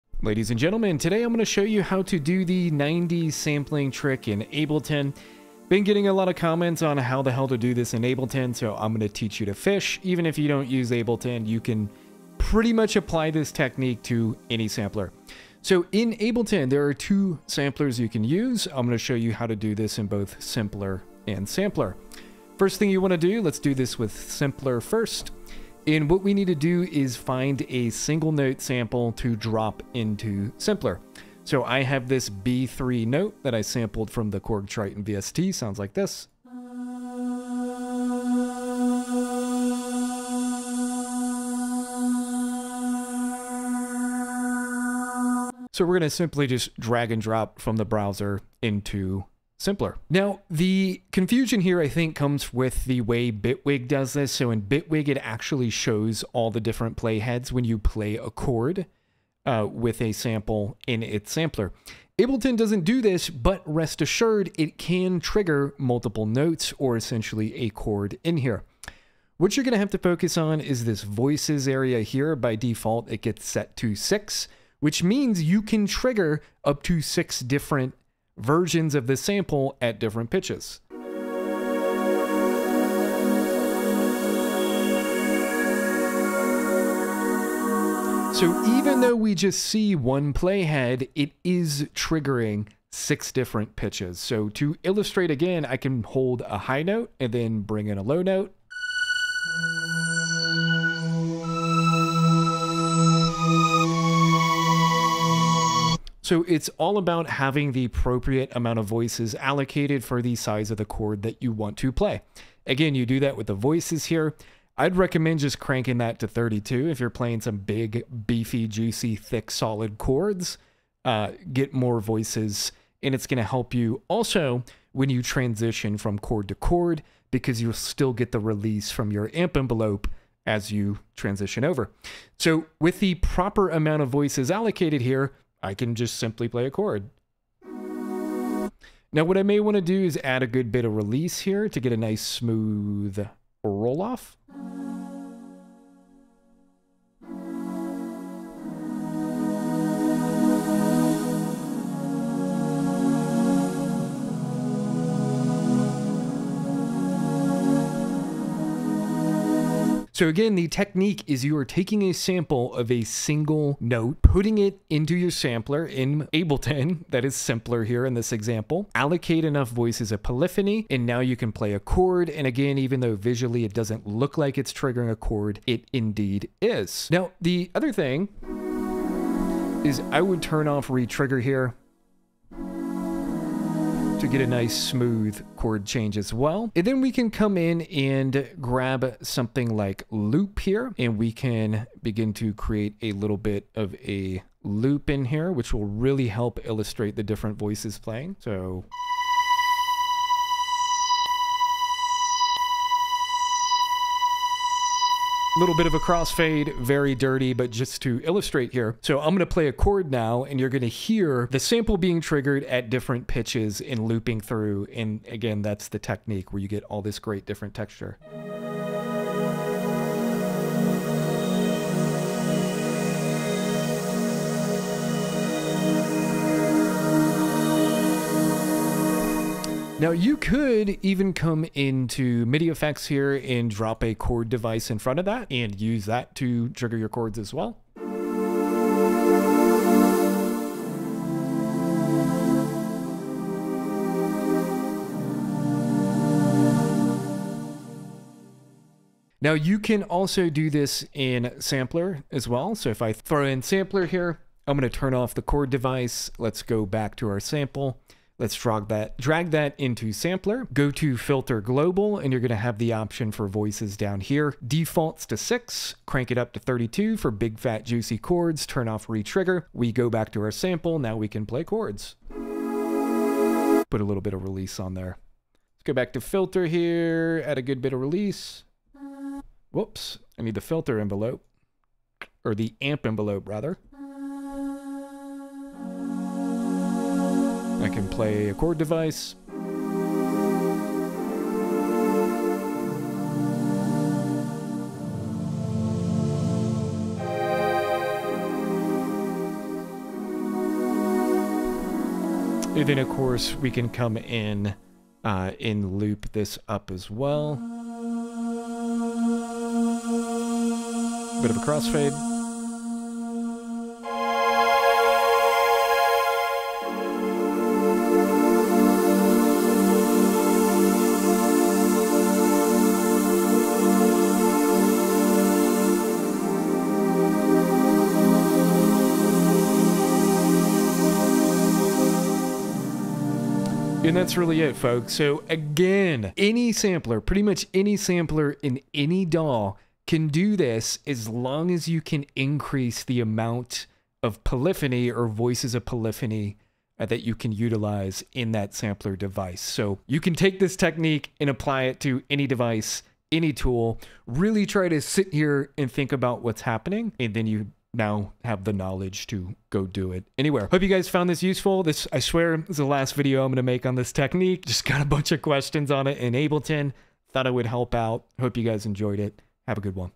the 90s ambient pad trick in Ableton
how to make ethereal, 90s style ambient pads in Ableton using simpler & sampler, as requested. Original Video

0:00 Hello
0:15 How to do it in Simpler
4:10 How to do it in Sampler
5:34 The End